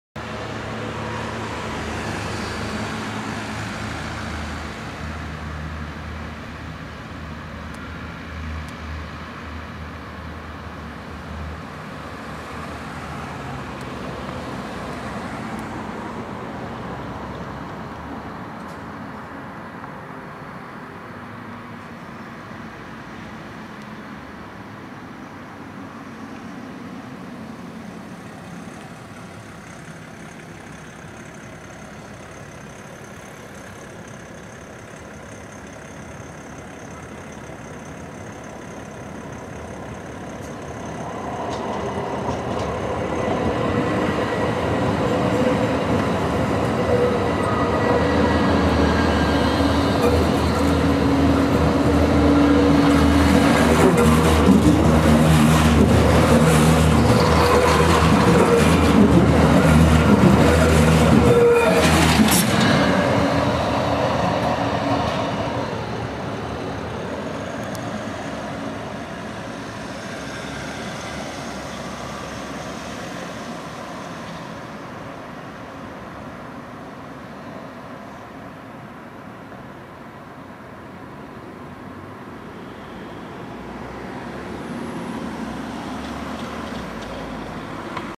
S4 Schwaigern West @ Heilbronn Finanzamt
Ein- und Ausfahrt der S4 nach Schwaigern West an der Haltestelle Finanzamt, mit GT8-100D/2SY-M.